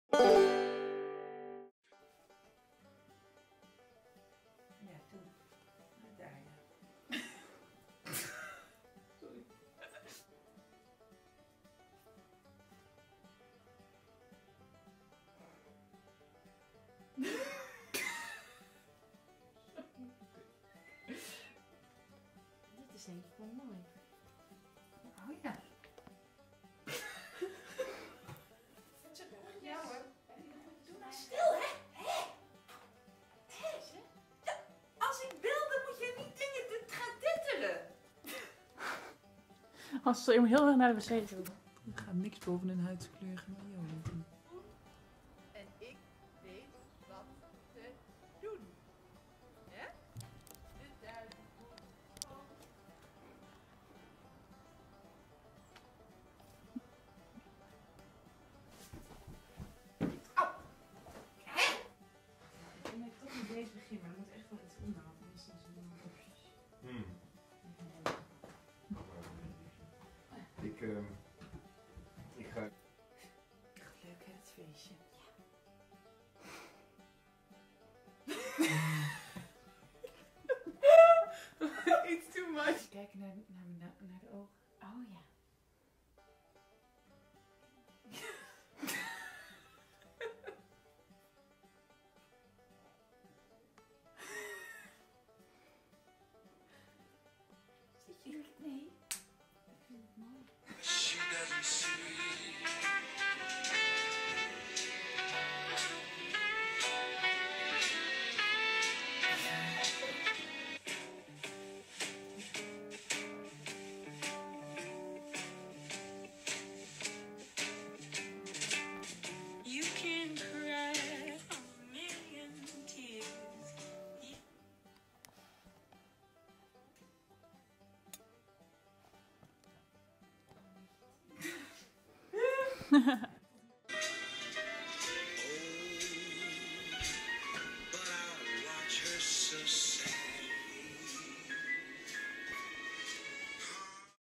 Ger en Marja - BLOOPERS
De afleveringen zijn volledig geïmproviseerd.

Ger en Marja, twee dames die worden gevolgd tijdens hun dagelijkse dingen.
Concept en spel:        Anne Freriks en Robin Kuiper
Camera en montage: Merel Oenema